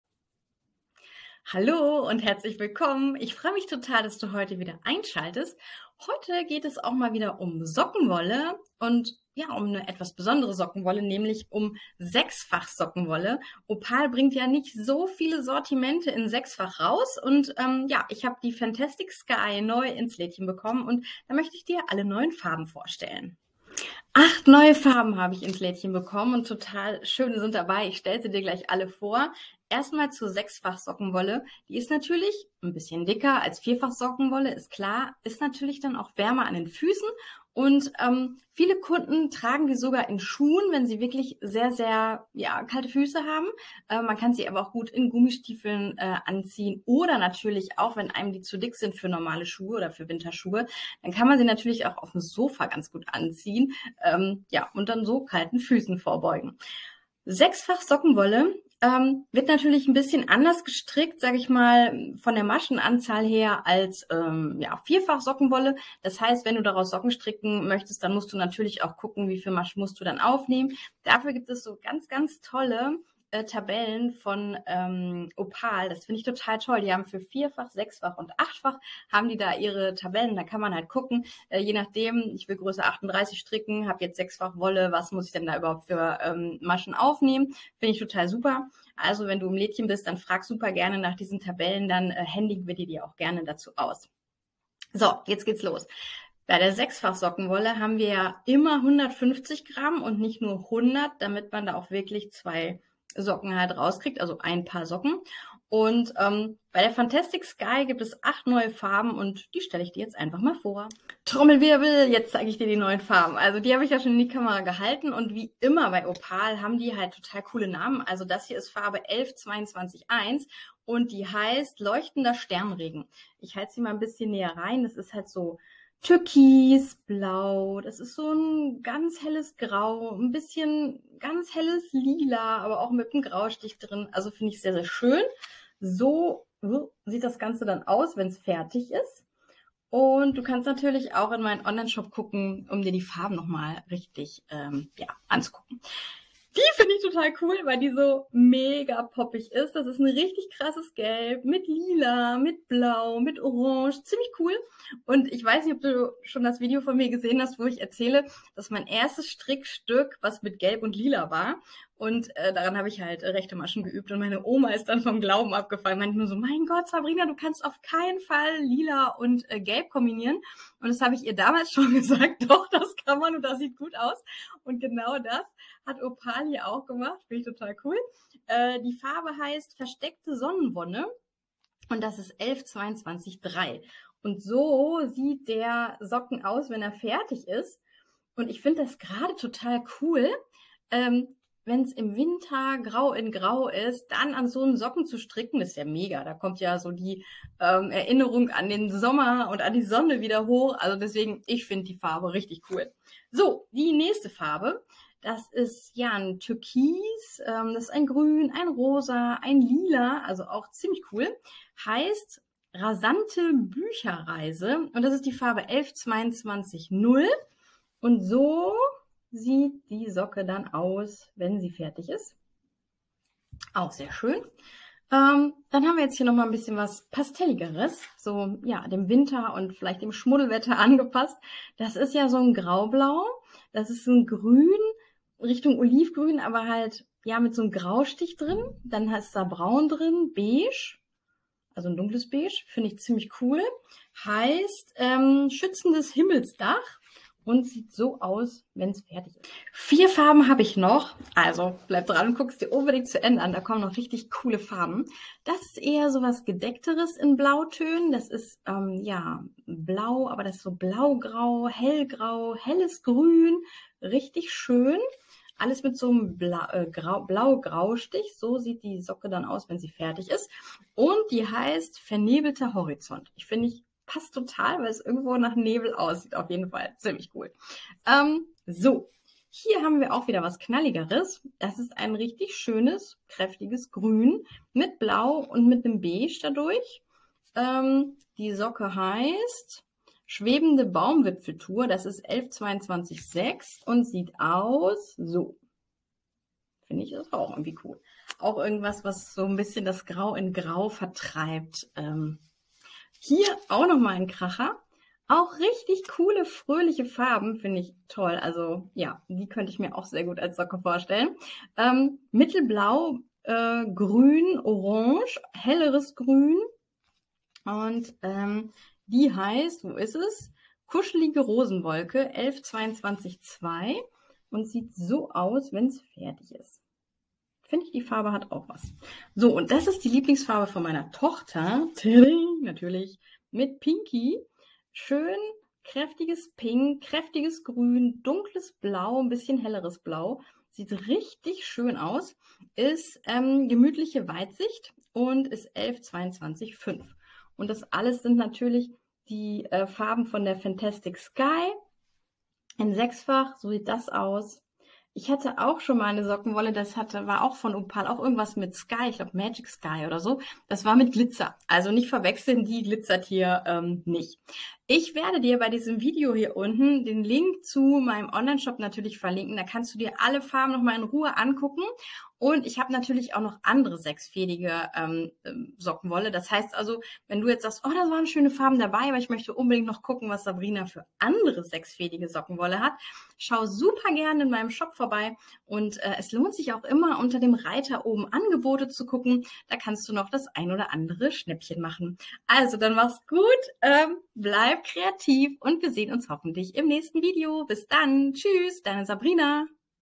Fantastic Sky von Opal - neues 6-fach Sockenwoll-Sortiment
Es ist wieder soweit. Es gibt ein neues 6-fach Sortiment von Opal. Die acht neuen Farben der Fantastic Sky stelle ich dir in diesem Video vor.

11220 - Rasante Bücherreise
11221 - Leuchtender Sternenregen
11222 - Kuschelige Rosenwolke
11223 - Versteckte Sonnenwonne
11224 - Schützendes Himmelsdach
11225 - Gemütliche Weitsicht
11226 - Schwebende Baumwipfeltour
11227 - vernebelter Horizont

Hier findest du alle 6-fach Sockenwolle, die ich aktuell am Lager habe.

Wünsche dir viel Freude beim Handarbeiten.
Wollige Grüße

Sabrina Karlem

Möchtest du jeden Monat eine Gratisanleitung erhalten, dann melde dich bei meinem wolligen Telegramm an

Das erwartet dich in diesem Video:

00:00 Herzlich willkommen
00:23 Die neuen Farben der Fantastic Sky (4 Farben)
04:10 weitere 4 Farben der Opal Sockenwolle